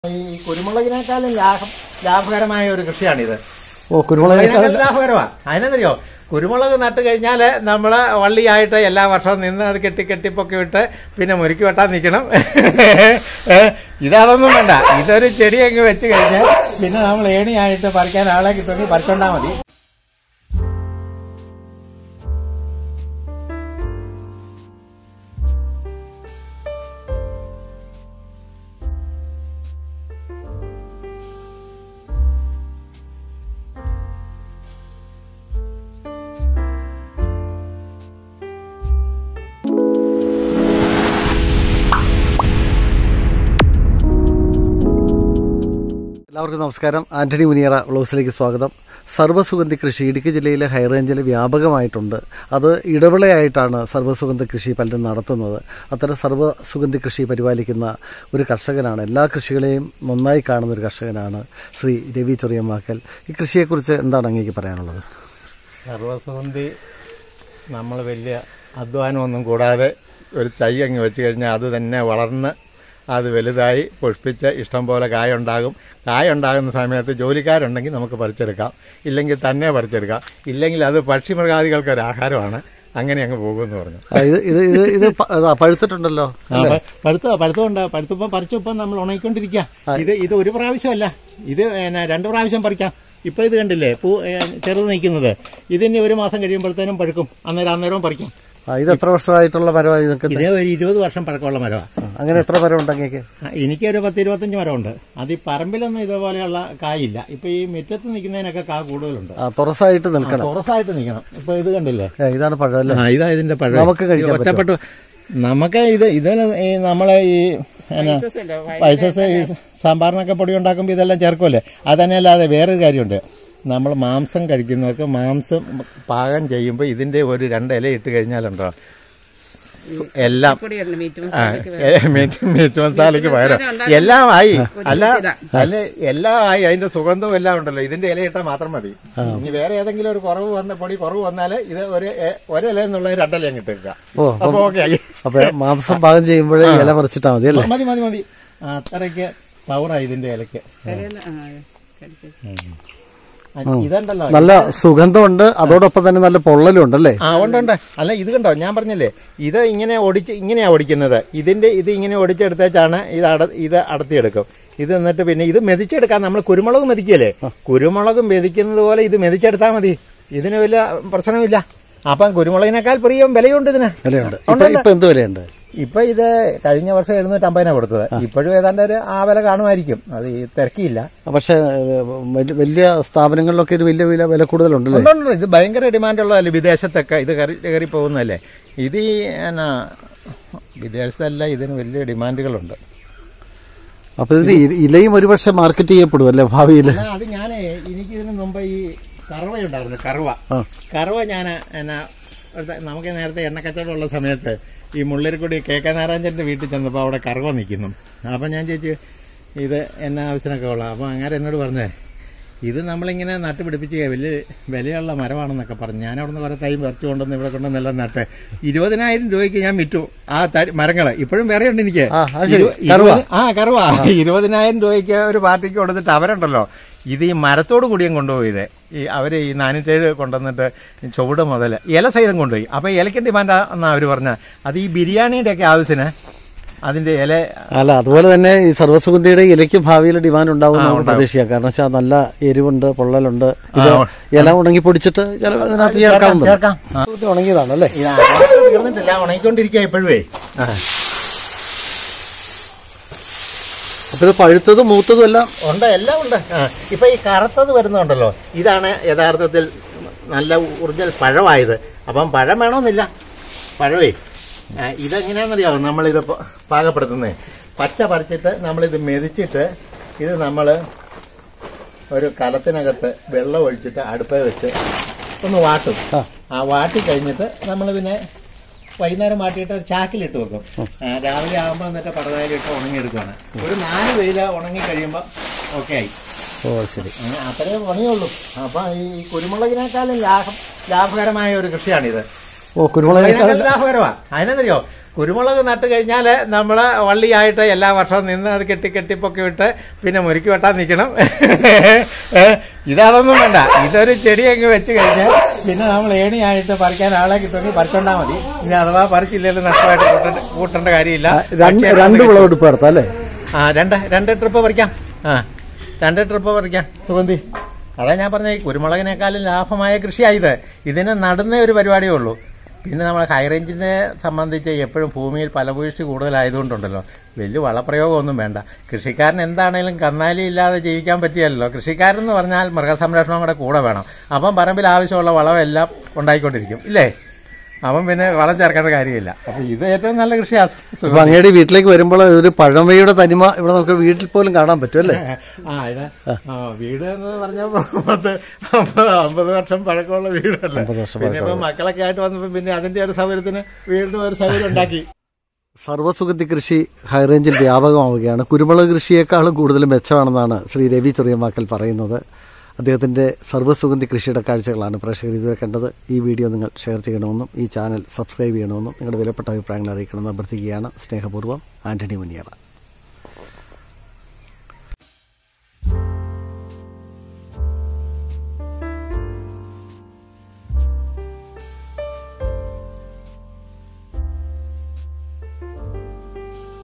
Sarva Suganthy (All Spices)antony muniyara VIogs
സർവ്വസുഗന്ധി നല്ല ആദായം തരുന്ന സുഗന്ധവിളയാണ് ഈ കൃഷിയിൽ നന്നായി ശ്രദ്ധിക്കുന്ന കർഷകനാണ് ഇടുക്കി കൊന്നത്തടി പഞ്ചായത്തിലെ മുനിയറ ചുരുളി സ്വദേശി സി.ജി.രവീന്ദ്രൻ ചൊറിയമ്മാക്കൽ